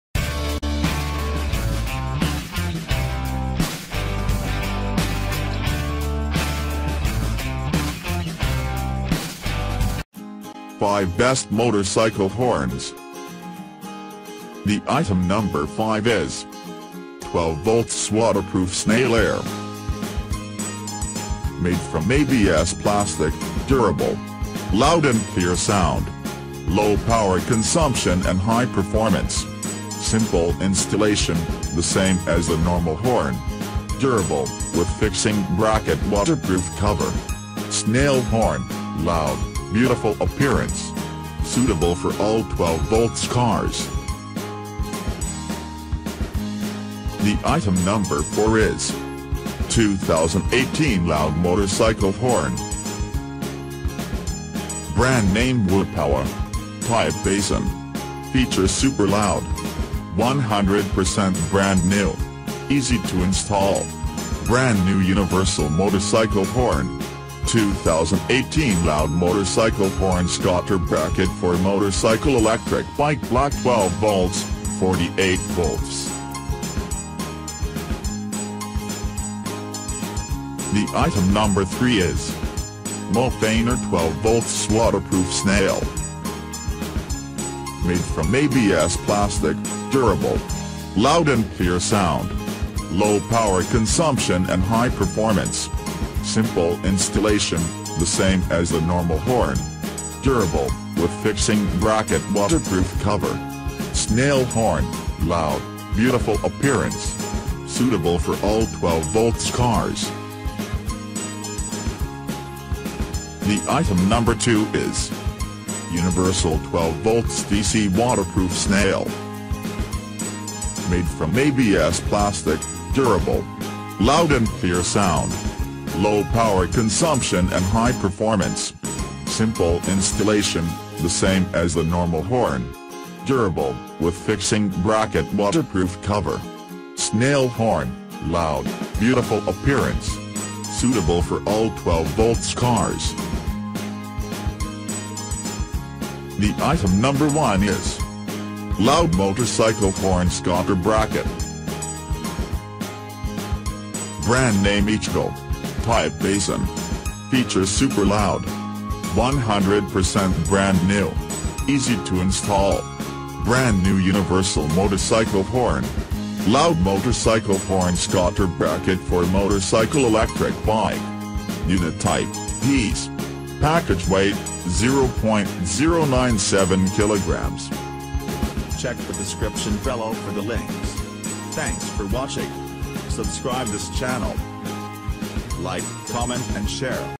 5 Best Motorcycle Horns
5 Best Motorcycle Horns

5.12V Waterproof Snail Air Motorcycle Horn Siren Loud 130dB For Truck Motorbike

4.2018 Loud Motorcycle Horn Scotter Bracket for Motorcycle Electric Bike Black 12V, 48V

3.Mofaner 12V Waterproof Snail Air Motorcycle Horn Siren Loud 130dB For Truck Motorbike

2.Universal 12V DC Waterproof Snail Air Motorcycle Horn Siren Loud 130dB for Truck Motorbike red gold blue silver black

1.Loud Motorcycle Horn Scotter Bracket for Motorcycle Electric Bike